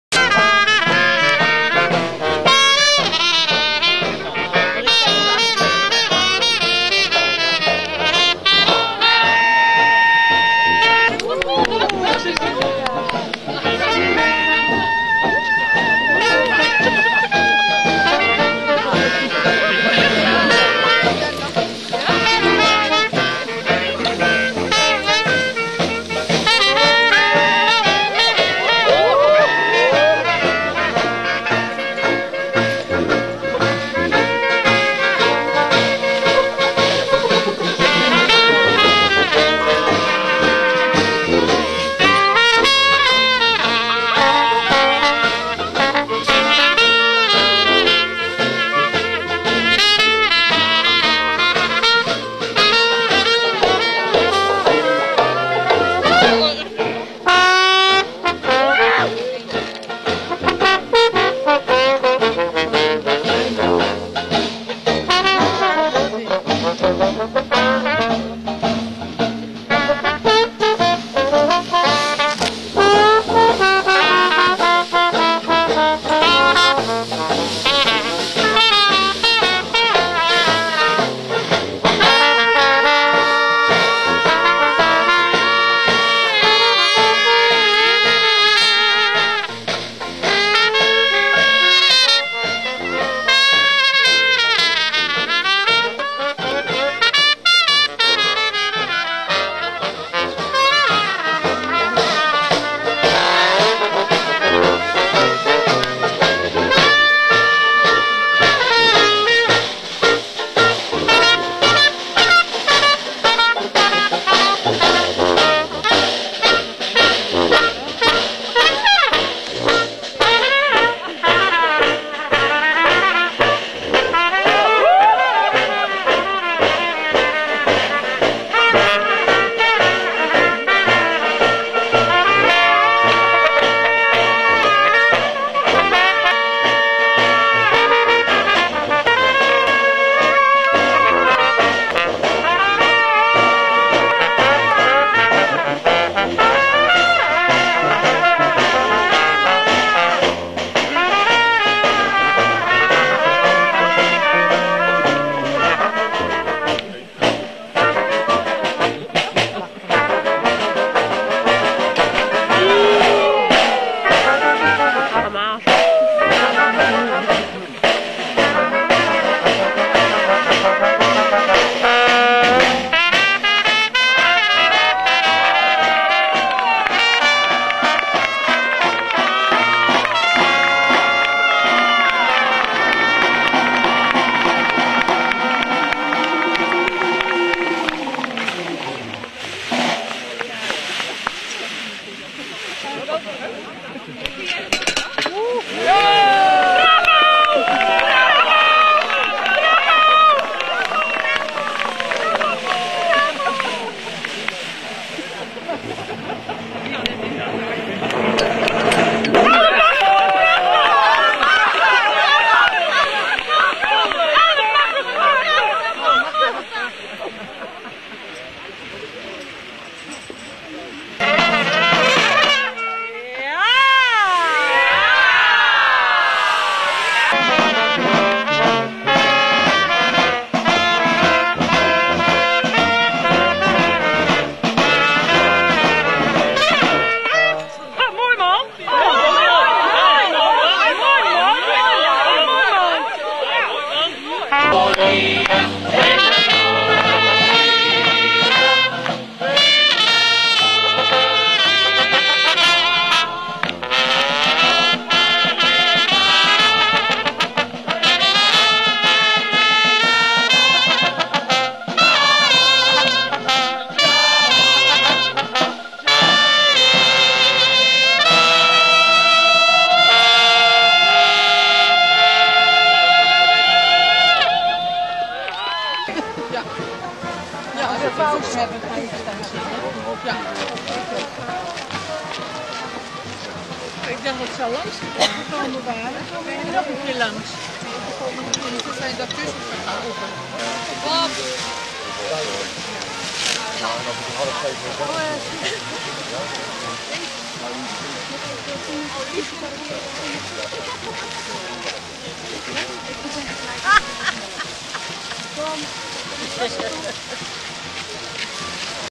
Eissingeheem Zandeweer
Eissingeheem Auth & Tiek, Verhildersum part 2. Beeldend theatergroep Auth & Tiek is bekend geworden door de theatervoorstellingen die op locatie gemaakt zijn, speciaal voor dié plek!